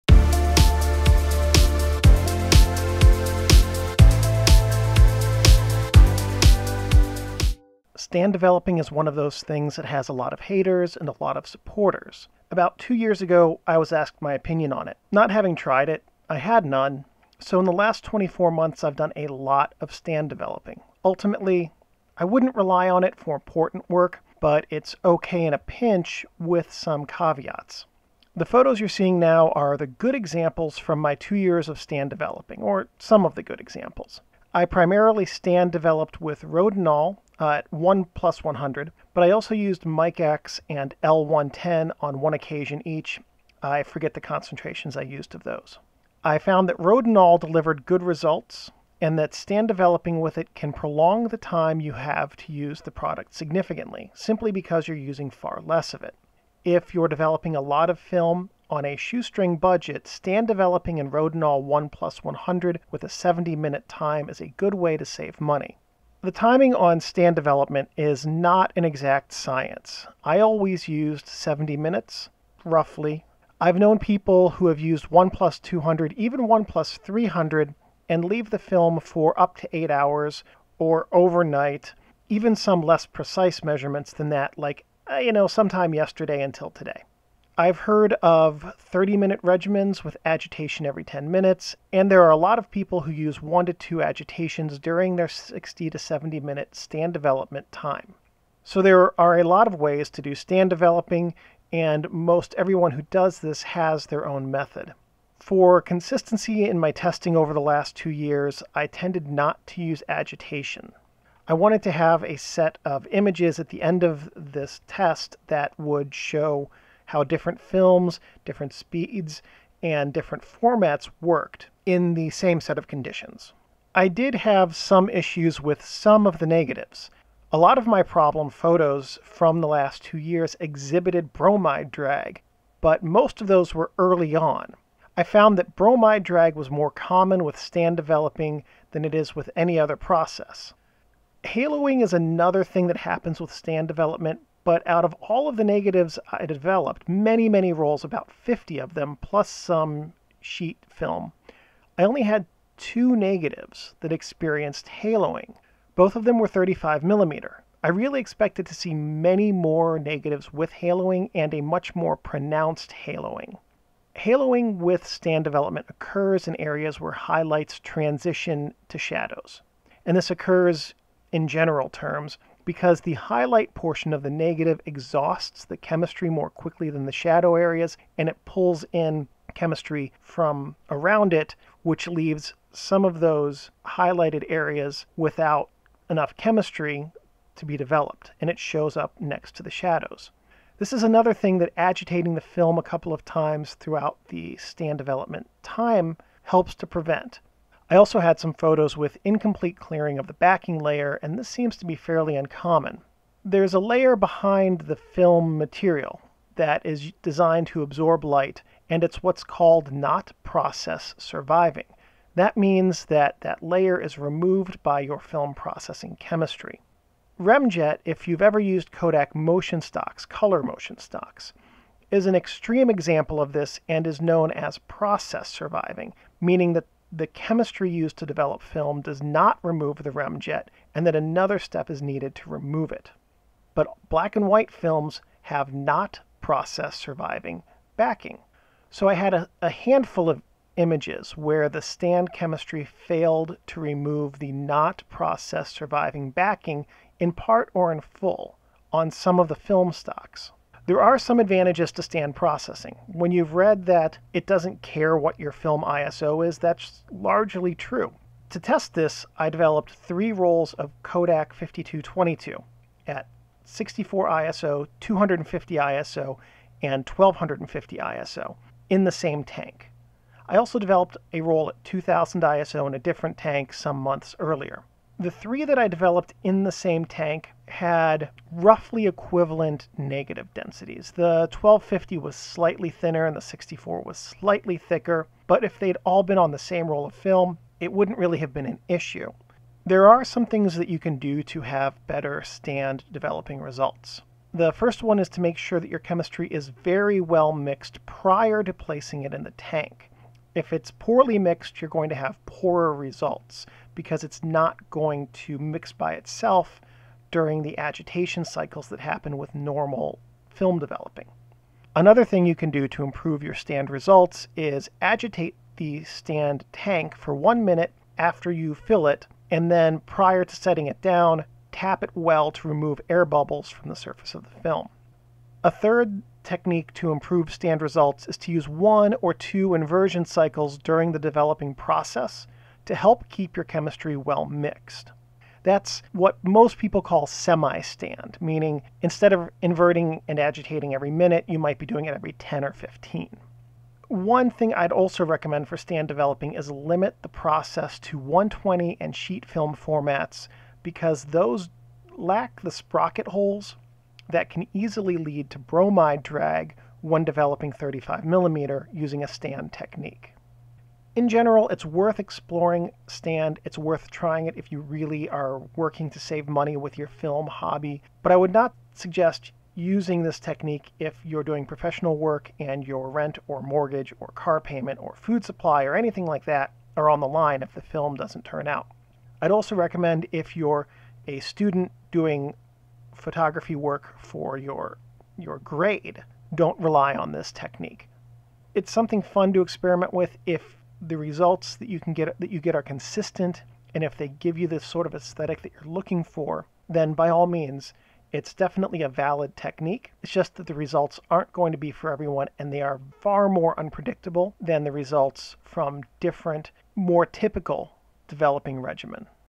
Stand Developing Guide: Sample Photos, Expectations, Avoiding Problems
Stand developing is one of those things that has a lot of haters and a lot of supporters. About two years ago, I was asked my opinion. Not having tried it, I had none. So, in the last 24 months, I’ve done a LOT of stand developing. Ultimately, I wouldn’t rely on it for important work, but it’s okay in a pinch, with some caveats.

The photos you’re seeing now are the good examples from my two years of stand developing. I primarily stand developed with Rodinal but also with Mic-X and L110, on one occasion each. I found that Rodinal delivered good results and that stand developing with it can prolong the time you have to use the product significantly. If you’re developing a lot of film on a shoestring budget, stand developing Rodinal at 1+100 for 70 minutes is a good way to save money.

The timing on stand developing is not an exact science. I always used 70 minutes. I’ve known people who use 1+200 and leave the film for eight hours. I’ve heard of 30-minute regimens with agitation every 10 minutes. So there are a lot of ways to do this. I tended to use no agitations for consistency in my tests.

I did have some issues with some negatives. These exhibit bromide drag. A lot of my problem photos had bromide drag. I found that bromide drag was more common with stand developing than with other developing processes. And that stands to reason because the bromide is slightly heavier than the chemistry and flows down the film’s surface in the direction of gravity. That requires time and agitation prevents that from occurring.

I only had two negatives exhibit haloing, out of the maybe 50 rolls of film and multiple 4X5 sheets that I developed. Both were 35mm, and I expected to see many more negatives halo. Haloing with stand developing occurs in areas where highlights transition to shadows. This occurs, in general terms, because the highlight areas exhaust the chemistry more quickly than the shadow areas. Agitating the film a couple of times tends to prevent this if you find it occurs often in your stand processed images.

I also had some photos with incomplete clearing of the backing layer. The layer behind the film is designed to absorb light and is not-process-surviving, meaning that it is removed by processing chemistry. Remjet, if you’ve used Kodak motion stocks, is an extreme example of this and is process-surviving, meaning that chemistry does not remove it and another step is needed. I have some images where the stand chemistry failed to remove the not-process-surviving backing of some films.

There are some things you can do to have better stand results.
- Make sure that your chemistry is well mixed prior to placing it in the tank.
- Agitate for one minute prior to starting and tap the tank well to remove air bubbles.
- Use one or two inversion cycles during the developing process to help keep the chemistry well mixed.
- Limit this process to 120 and sheet film formats because those lack the sprocket holes that can easily lead to bromide drag.

Off-camera Audio- Tascam DR-70D or Tascam DR-60D MKII and Tascam DR-05